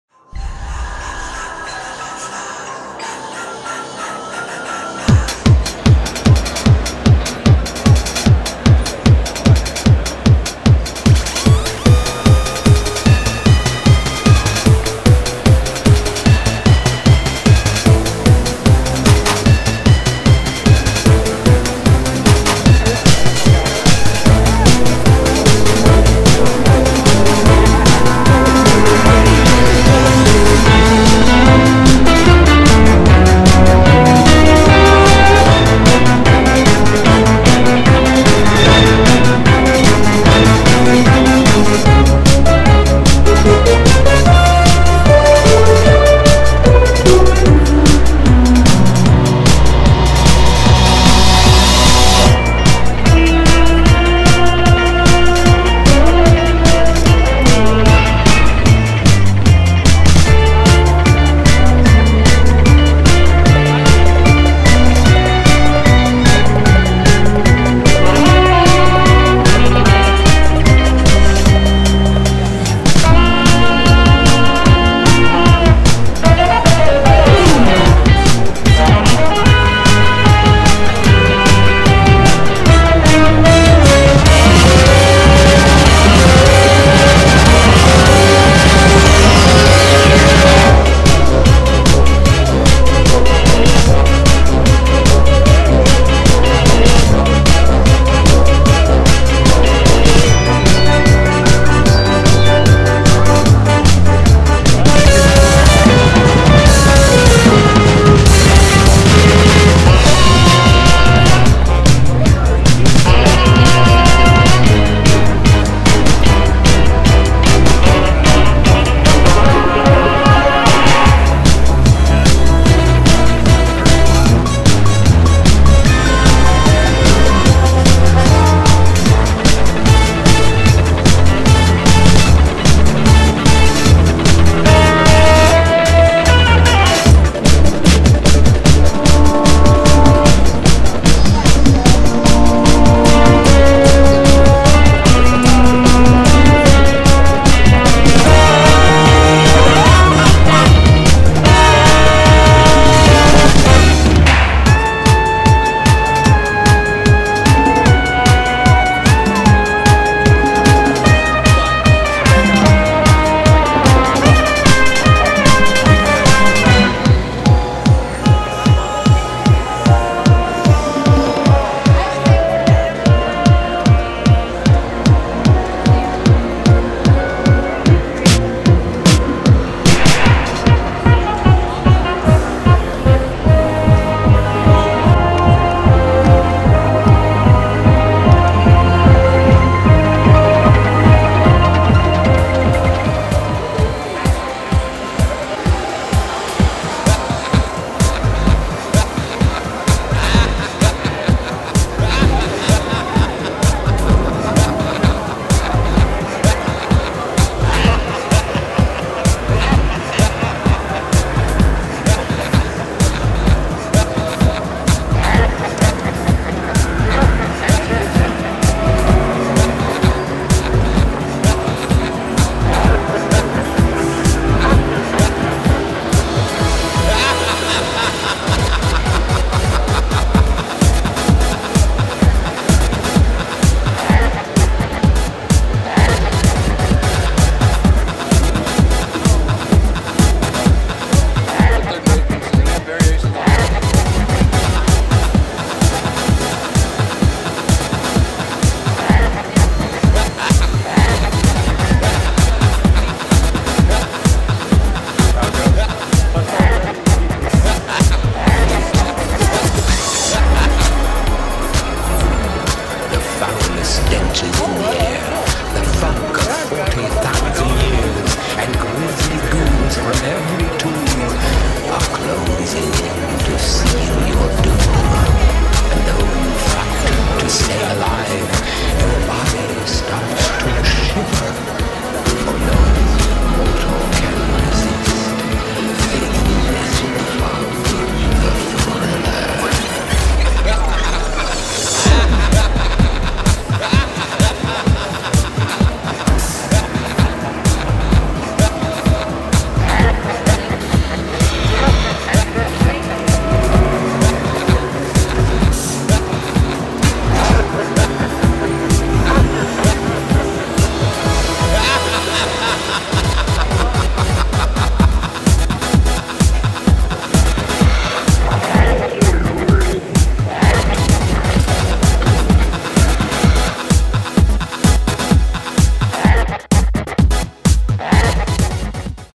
2010 Midwest Haunters Convention Show Floor
Here is a look at most of the vendors from the 2010 Midwest Haunters Convention Show Floor.  Masks, props, make-up, and more!